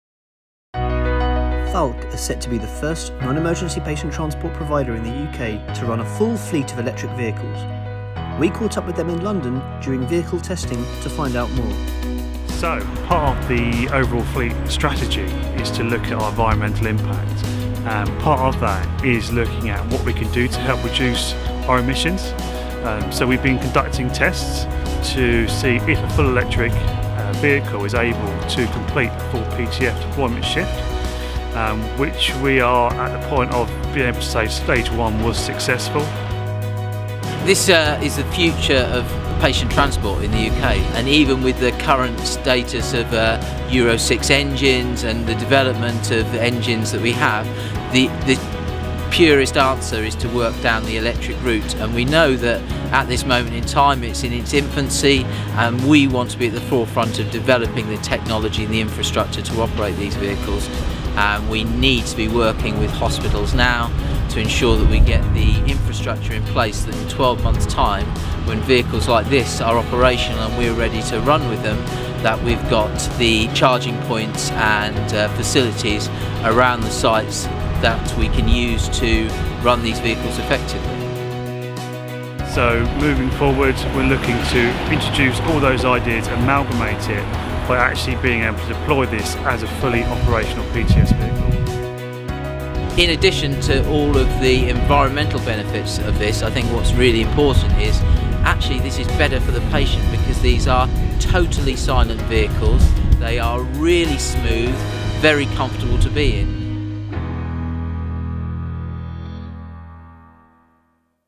Someone has to take the lead - trialling electric patient transport vehicles in London | Falck
At Falck we are committed to ensuring we deliver our services as sustainably as possible and where we can, we limit our carbon footprint.
This November our Patient Transport Teams in London, began an ambitious new trial of a fully electric Patient Transport vehicle to test whether it had the ability to safely and comfortably deliver a full day shift. See how we got on and hear about our vision for a sustainable Patient Transport Service in the future.